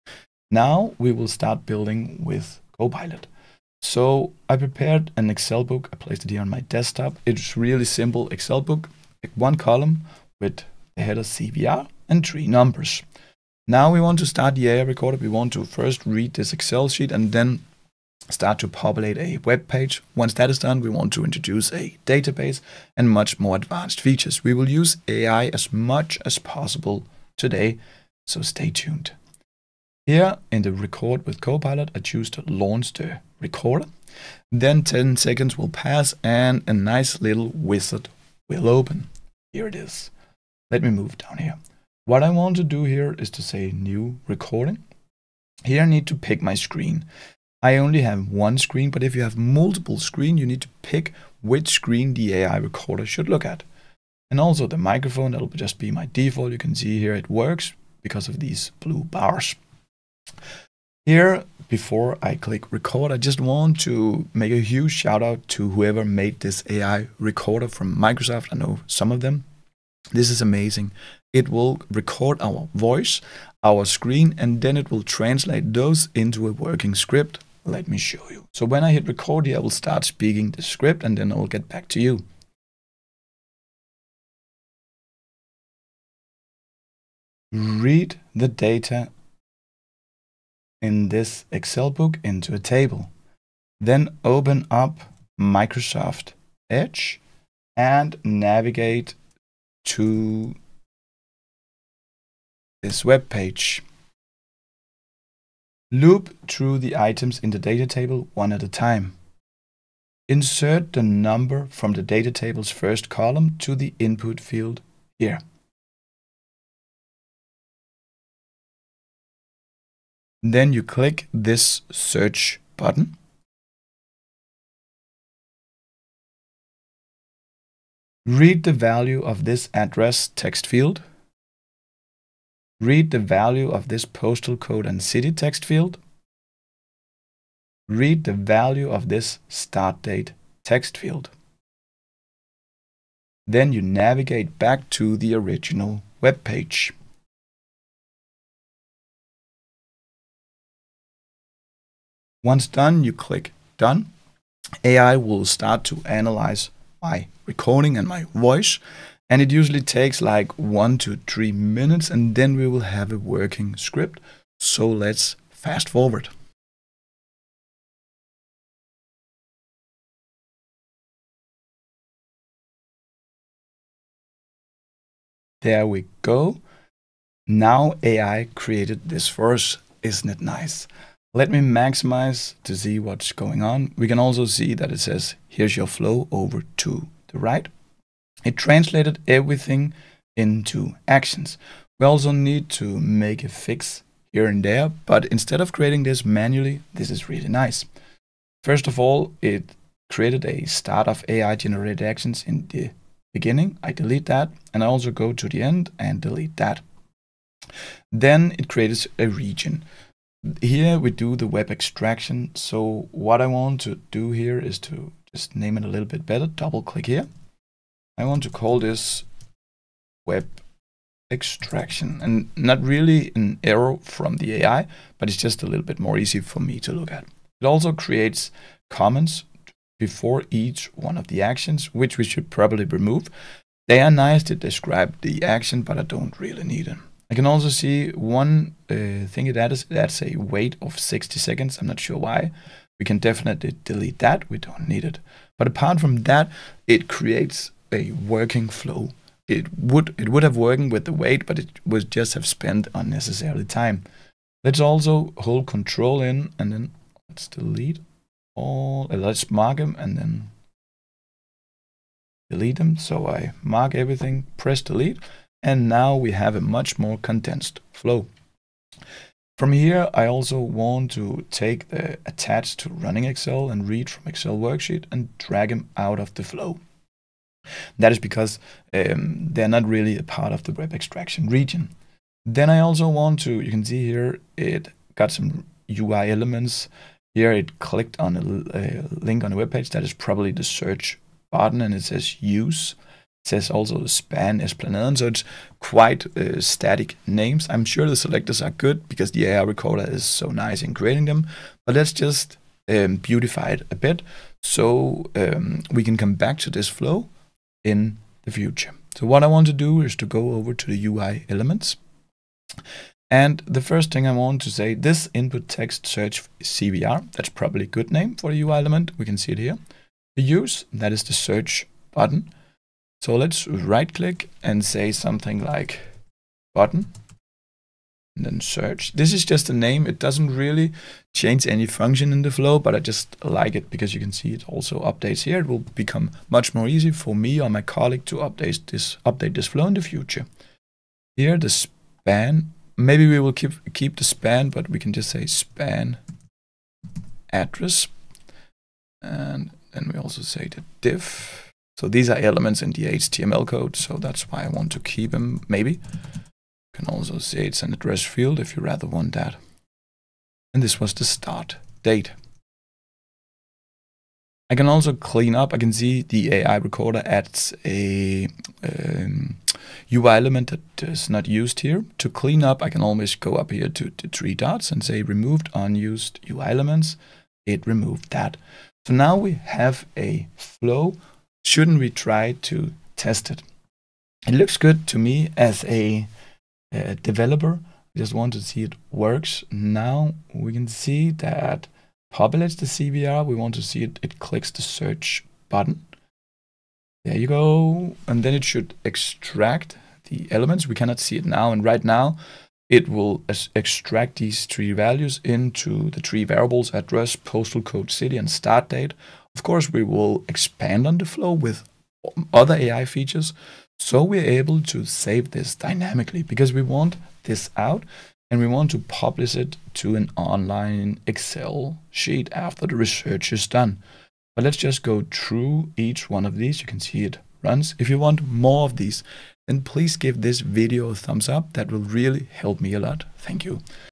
How to use the AI Recorder in Power Automate Desktop
In this video tutorial, you’ll learn how to use AI Recorder with Power Automate Desktop to automate tasks efficiently. We start by reading an Excel sheet and populating a webpage, then introduce a database for advanced automation. With Microsoft’s AI tools, we simplify workflows and optimize data handling.

TOP LEARNINGS
🔹 How to use AI Recorder with Power Automate Desktop
🔹 Automating Excel data extraction and web input
🔹 Optimizing workflows with AI-powered scripting

By the end of this video, you'll know how to automate repetitive tasks using AI Recorder and Power Automate Desktop. You’ll also learn how to refine automation scripts for better efficiency.